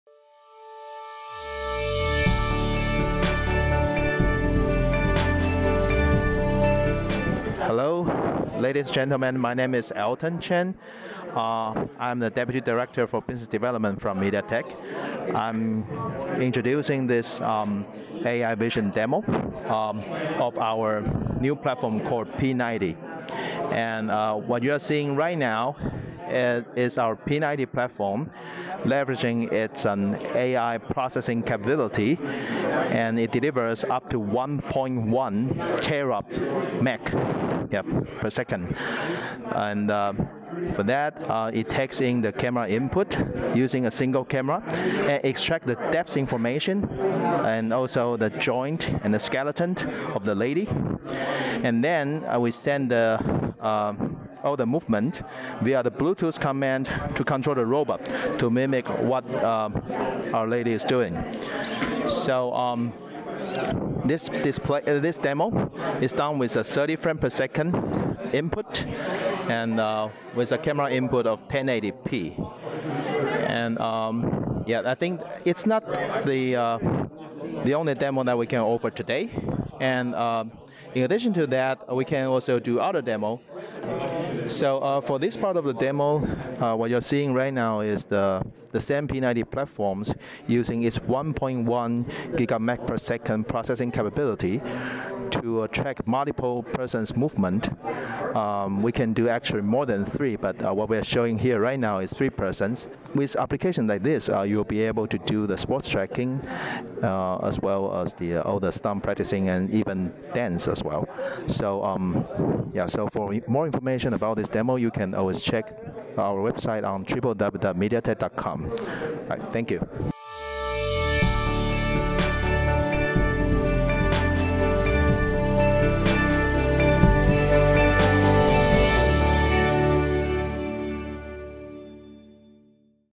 MediaTek Demonstration of Computer Vision Applications Using the Helio P90
Alton Chen, Deputy Director of Corporate Business Development at MediaTek, delivers a product demonstration at the March 2019 Vision Industry and Technology Forum. Specifically, Chen demonstrates various computer vision applications implemented on the company's Helio P90 application processor.

MediaTek's Helio P90 is an AI processing powerhouse, boasting the company’s latest AI Processing Unit (APU) 2.0. With a leading fusion AI architecture combining MediaTek’s in-house AI accelerator and an off-the-shelf DSP, the Helio P90 handles complex AI applications and tasks while running multiple AI functions simultaneously. The APU 2.0 supports 1165 GMACs, resulting in flagship SoC performance with a 50% increase in performance compared to high-end competitor devices. Its powerfully efficient APU boosts INT8 speeds by up to 20 percent, with greater power efficiency.